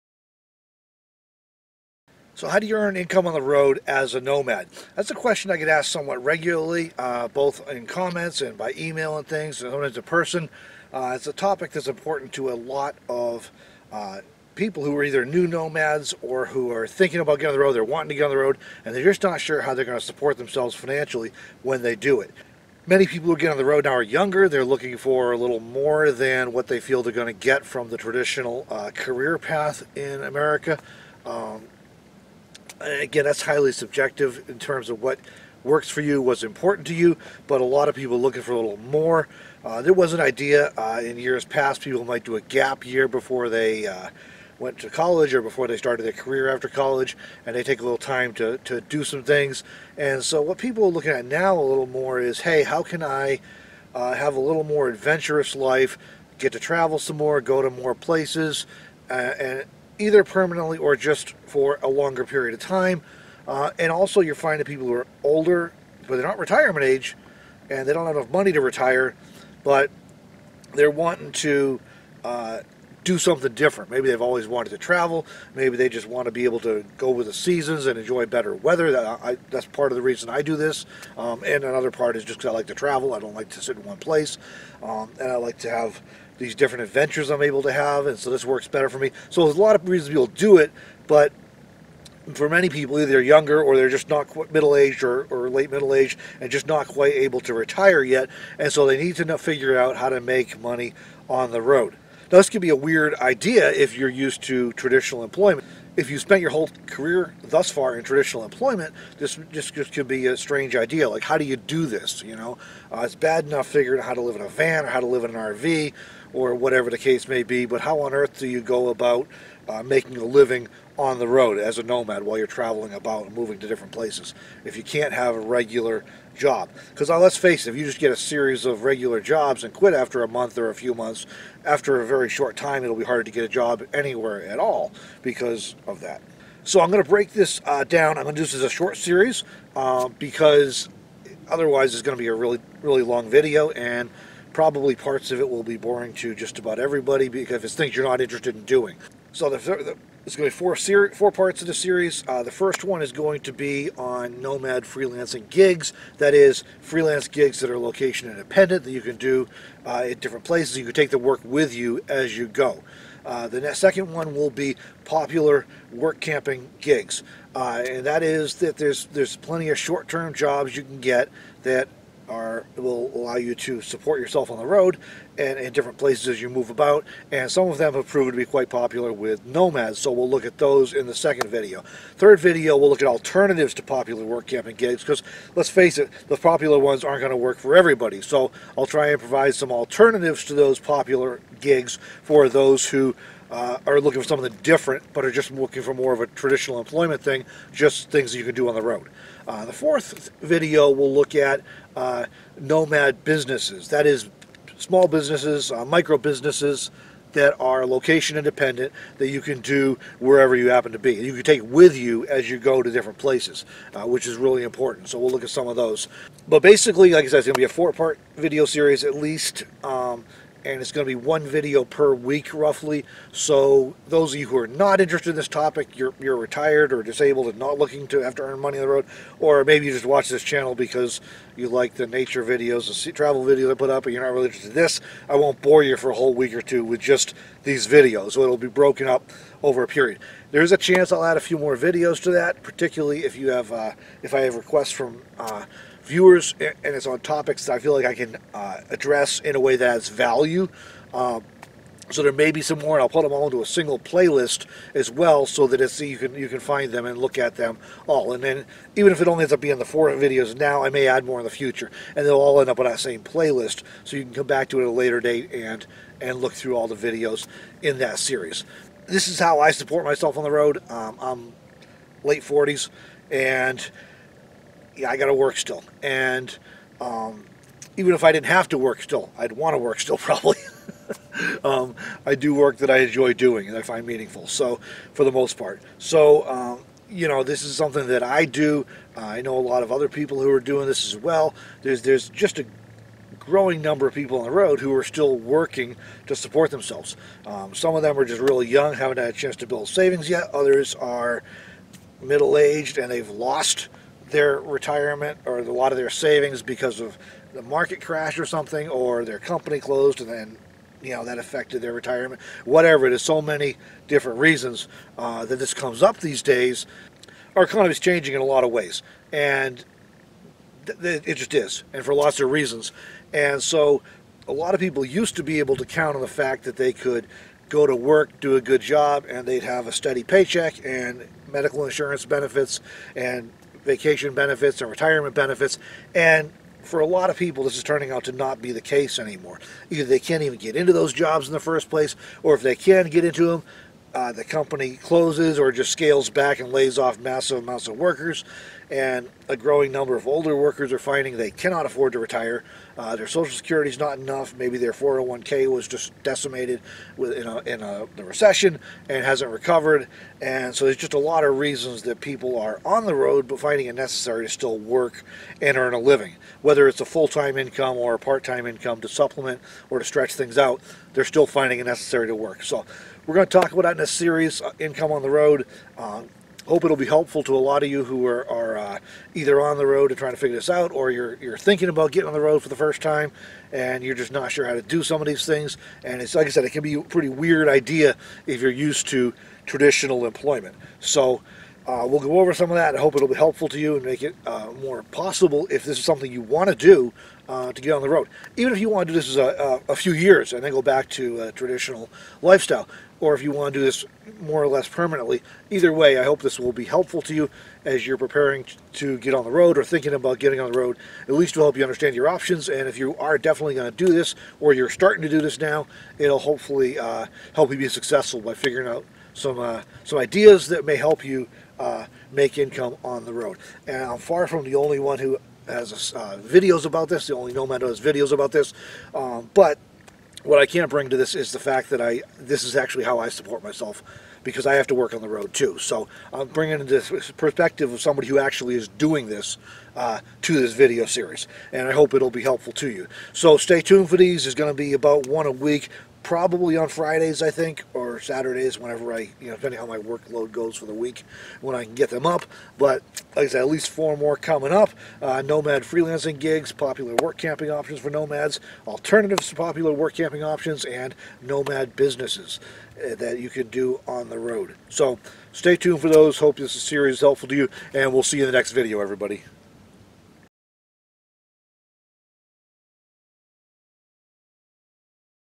Income on the Road - Introduction
I am frequently asked how travelers earn income on the road. This is the first video in a short series that will address this important question. After all, many people who are becoming snowbirds or travelers still need to earn income.
- Freelancing Gigs for Nomads
- Popular Work Camping Jobs for Nomads
- Alternatives to Popular Work Camping Jobs for Nomads
- Microbusinesses for Nomads

*SOCIAL MEDIA*
Robert Witham